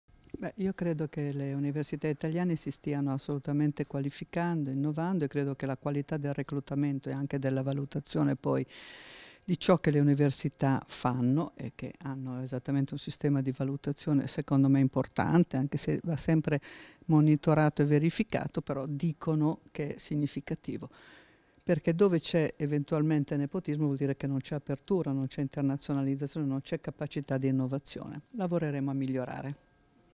Nepotismo all'Università, Fedeli: "Miglioreremo ancora"
(Agenzia Vista) - Roma, 4 Luglio 2017 - La Ministra dell'Istruzione, dell'Università e della Ricerca Valeria Fedeli a margine del convegno "Scuola digitale e cultura diffusa", promosso dalla Commissione Cultura della Camera.